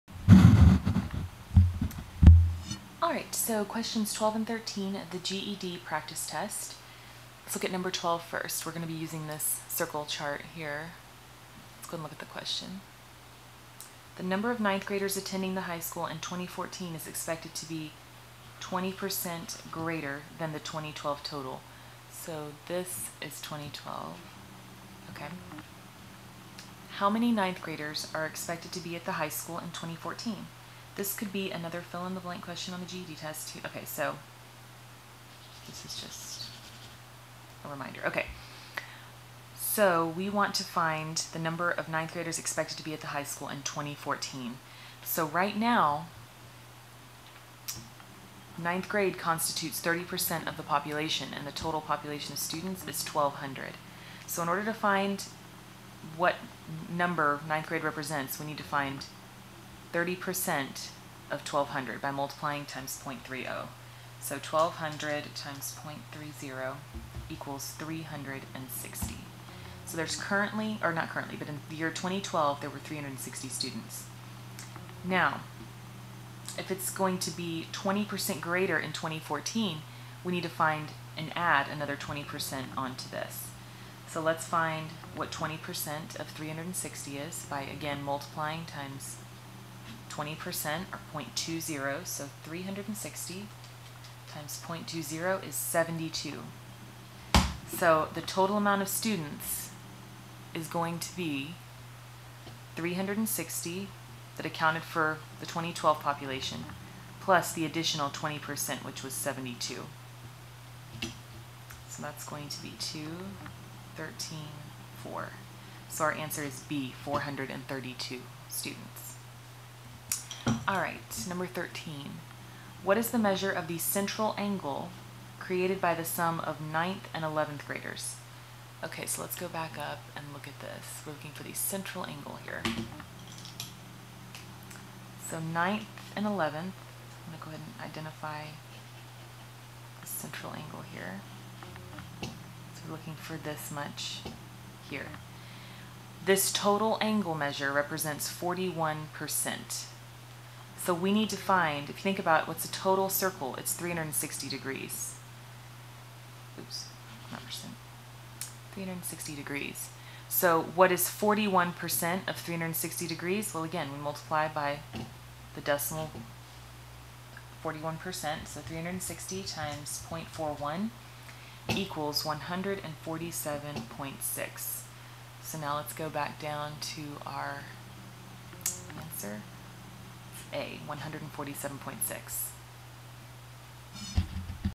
GED Practice Test Problems 12 and 13
FREE GED Math Practice Test problems worked step by step! MathTutorApril.com
Problems 12 and 13. Reading a circle graph, pie chart, evaluating percentages, calculating central angle based on percentage.